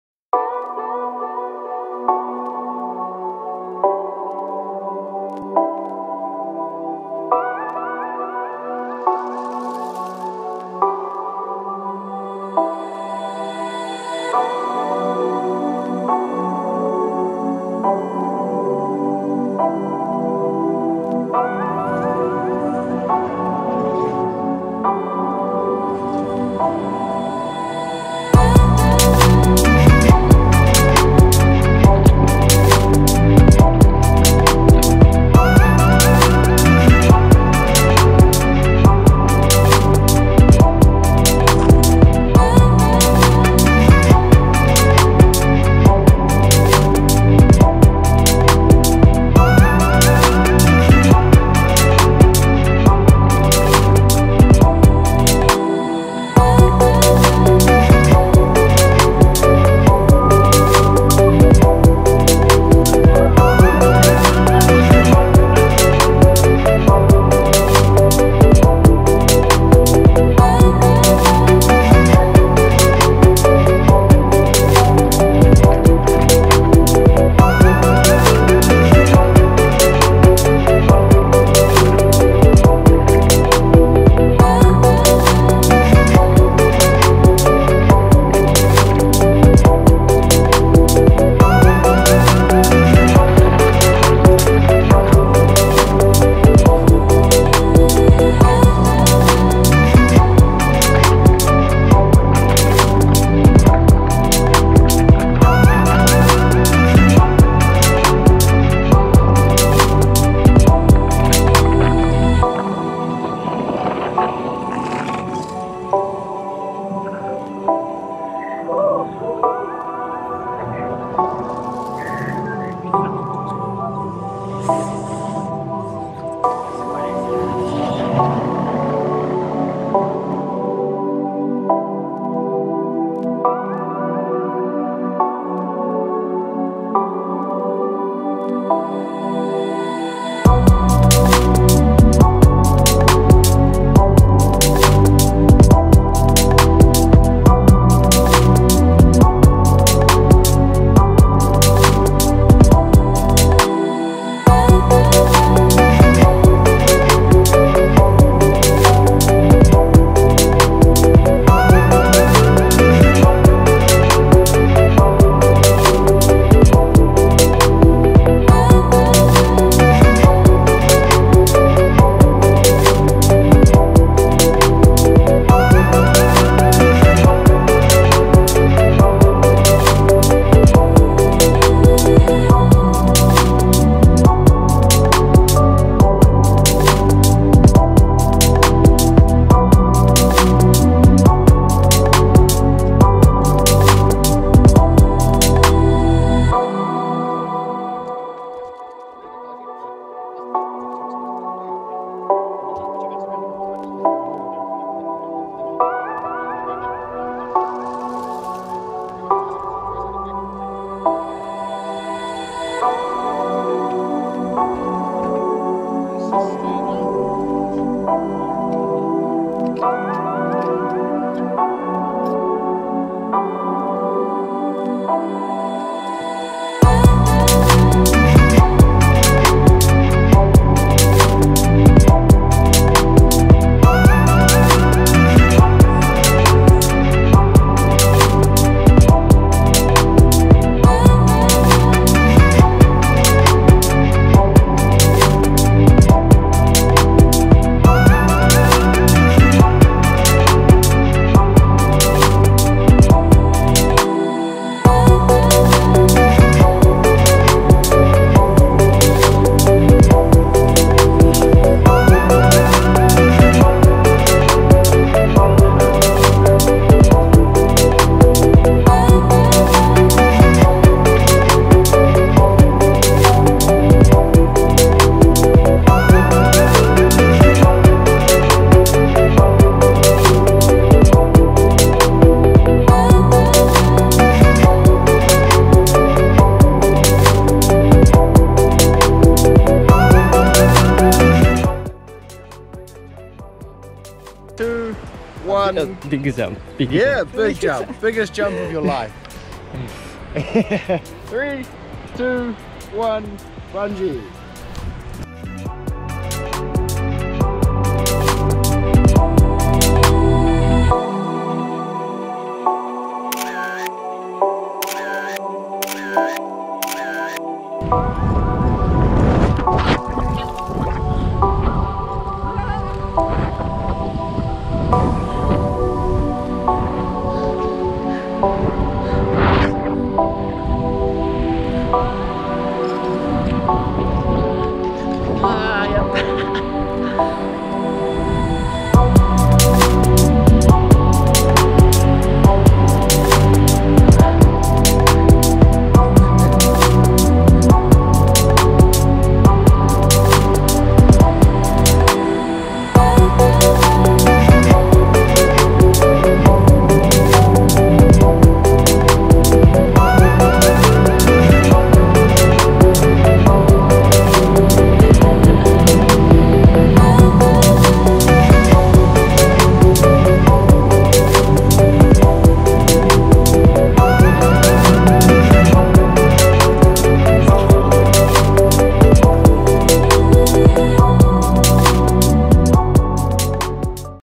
Bridge Bungy Jump & Jet Boat in Hanmer Springs, New Zealand | バンジージャンプとジェットボート！
(🇯🇵日本語は下の方にあります )

Had the most epic adrenaline-fuelled experience bungy jumping and jet boating in Hanmer Springs, New Zealand. Free falling 35 metres from a 135 year old historic bridge over the beautiful blue water below. There’s a reason Hanmer Springs Attractions is voted the #1 activity in Hanmer Springs!

Huge thanks to Hanmer Springs Attractions for giving us the opportunity to visit and film here! Check them out below 👇🏻

🇯🇵ニュージーランドはハンマースプリングスというところで、ジェットボートとバンジージャンプに挑戦させて頂きました。ジェットボートは最高時速70km程の猛スピードで絶景の中を疾走！スリルと感動が同時に味わえます！またバンジージャンプは美しい山と川の景色を存分に感じながらジャンプ！35mのフリーフォール！こちらもスリル満点！ハンマースプリングスアトラクション様ありがとうございました！ホームページは上のリンクからチェックして下さいね👆